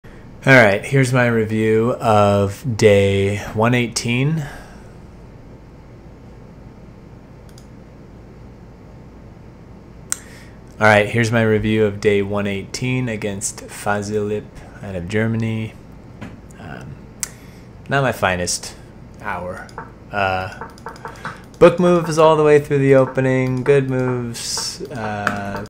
Analysis: Day 118 of my Journey to 2000
In this video I analyze Day 118 of my Journey to 2000. Hopefully by reflecting on my chess games I will be able to be a chess master :)

66% Accuracy